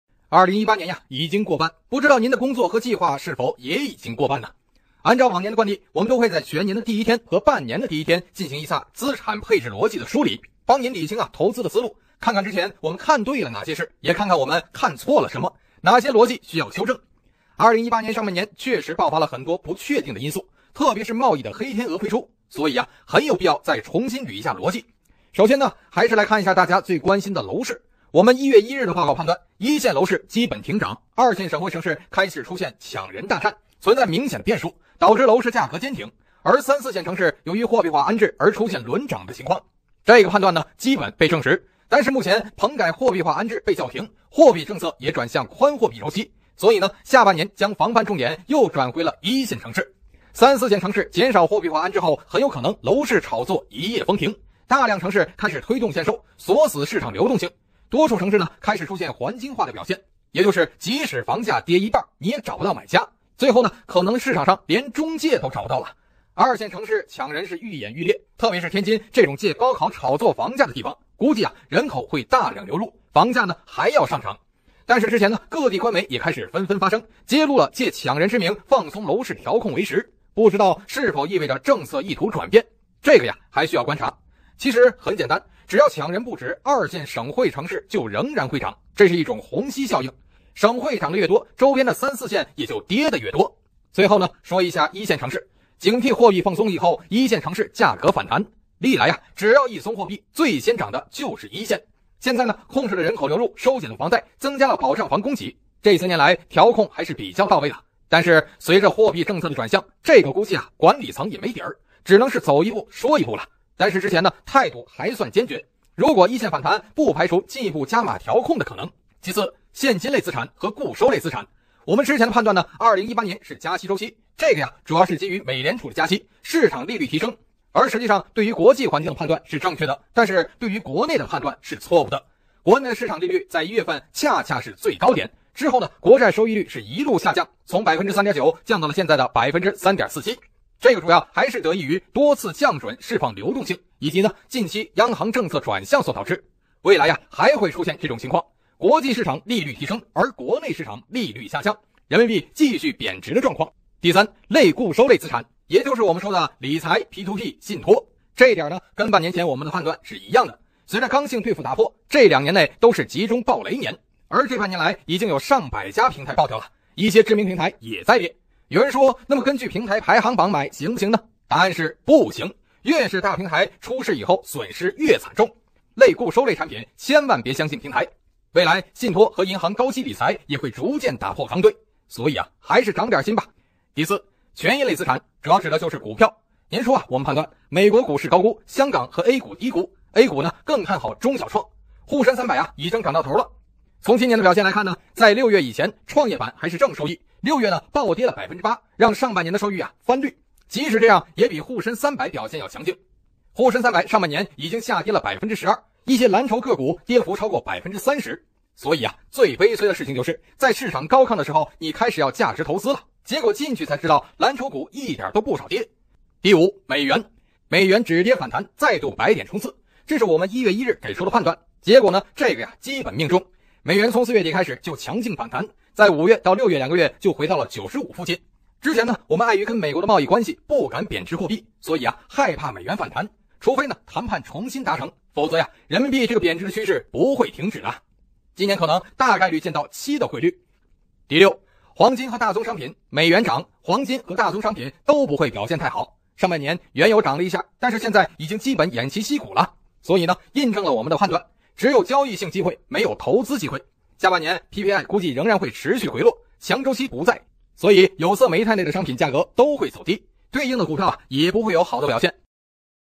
多行業岌岌可危 下半年中國投資者的資產何處安放
2018年已經過半，不知道您的工作和計畫是否也已經過半，按照往年的慣例，幫您理清投資的思路。看看之前我們看對了哪些事，也看看我們看錯了什麼，哪些邏輯需要修正。2018年上半年，確實爆發了很多不確定的因素，特別是貿易的黑天鵝飛出，所以很有必要再重新捋一下邏輯。